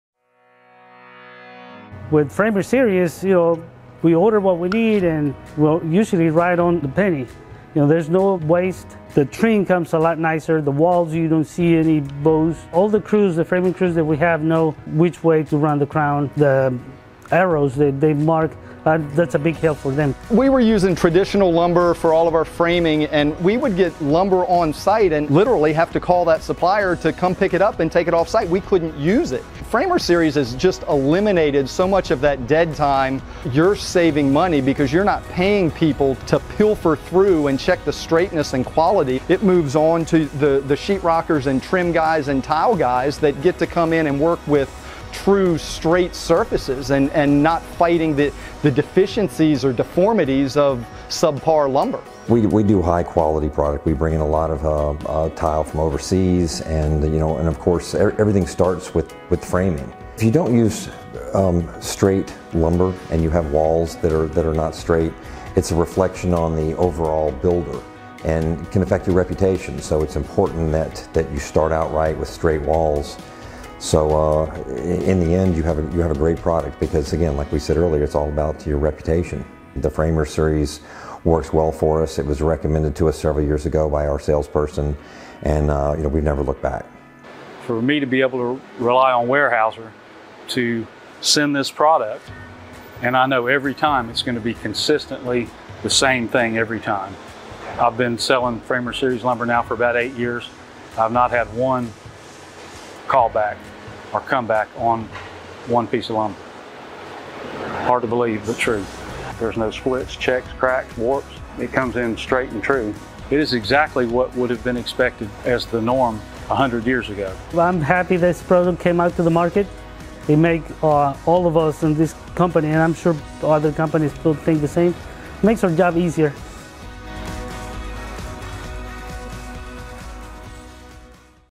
Framer Series Lumber - The White Sands Contracting Experience
From the dealer to the framer to the owner, Weyerhaeuser Framer Series Lumber adds value and peace of mind at every step of the building process.